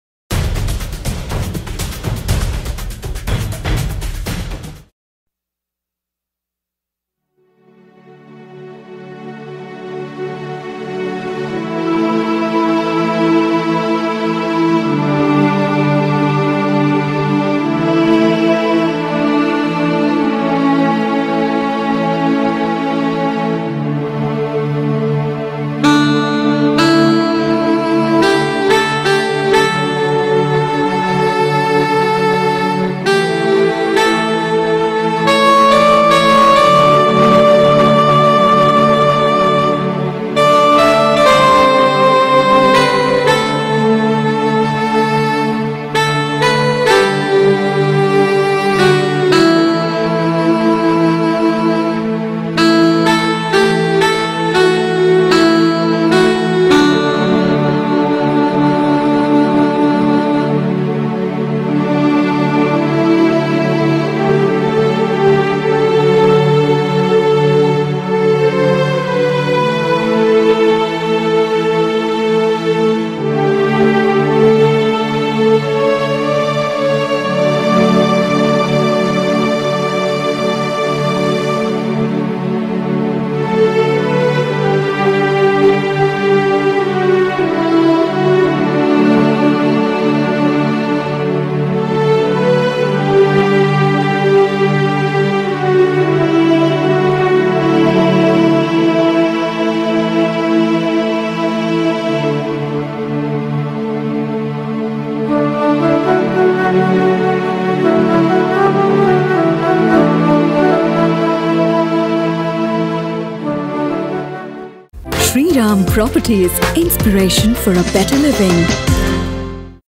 Unsung Heroes of the Indian National Movement
In this age of globalisation the heroes who gave us our freedom are hardly remembered. Of course there are the occasional Indian Film blockbusters like Rang de Basanti and Lage Raho Munnabhai that instils in a sense of pop-patriotism. Gandhi and Nehru continue to be cool, but for the Gen-next, with nano second attention spans, the freedom fighters are mere academic pages. However, each freedom fighter has his and her own story to tell. The following is a very small selection, some of them less eulogised, from the long list. Watch this video to know about them.